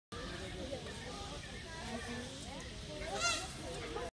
Beautiful peacock
A sneak peak of my trip to Mysore🙊

Will post full vlog in few days 🤪

Video credit- Samridhi sharma